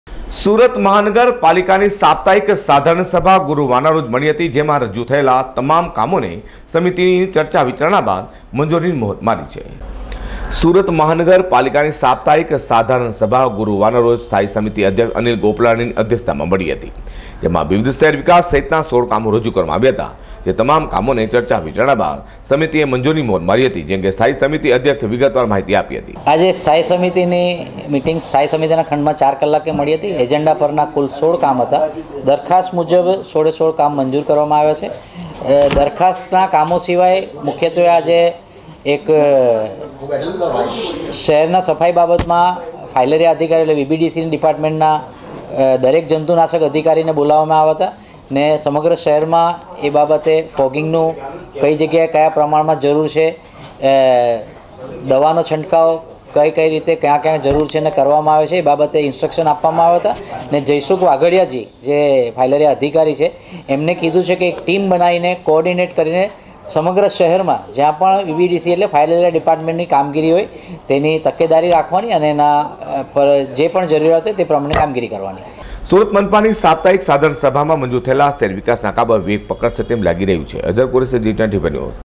standing briefing સુરત મનપાની સાપ્તાહિક સાધારણ સભા મળી
સભામાં રજુ થયેલા તમામ કામોને મળી મંજુરીની મહોર (સુરત - મુગલી